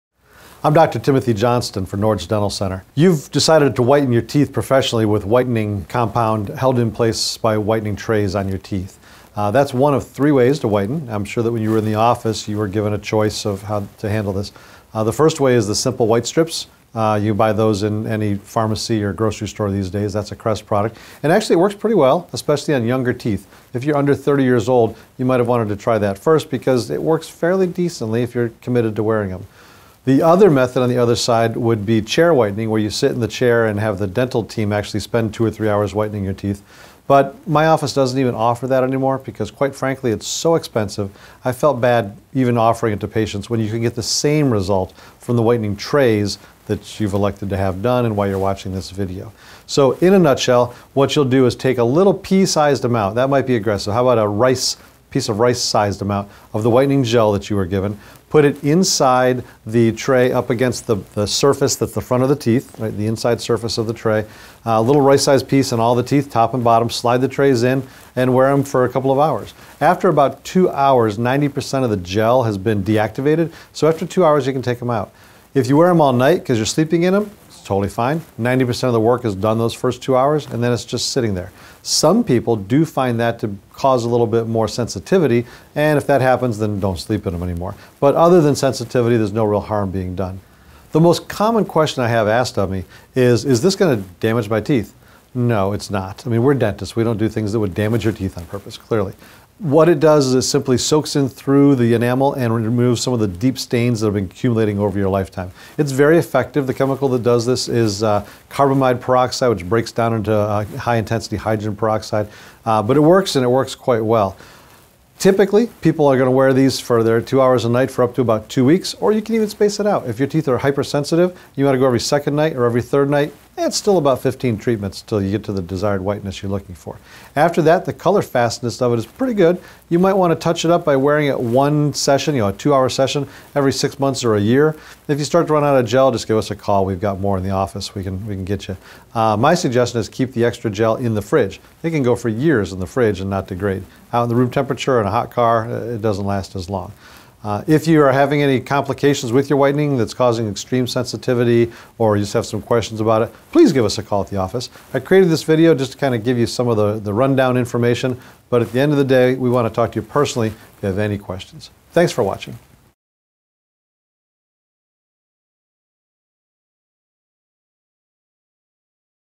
Whitening Trays - Introduction
Dr. Timothy Johnson explains the basics of whitening trays.